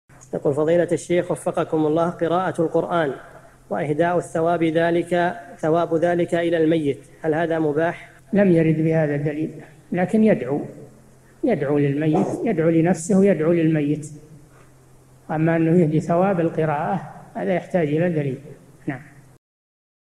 [363 /1350] قراءة القرآن وإهداء ثواب ذلك إلى الميت هل هذا مباح؟ الشيخ صالح الفوزان
-[363 /1350] السؤال: قراءة القرآن وإهداء ثواب ذلك إلى الميت هل هذا مباح؟
 الجواب: لم يرد بهذا، لكن يدعو للميت يدعو لنفسه ويدعو للميت، أما أنه يهدي ثواب القراءة هذا يحتاج إلى دليل.
للاشتراك في قناة الشيخ صالح الفوزان
أسئلة وأجوبة من شرح قاعدة جليلة ( 1350 ) مقطع

للاستفسار والإقتراحات والدعم
رئيس الفريق
لمشروع كبار العلماء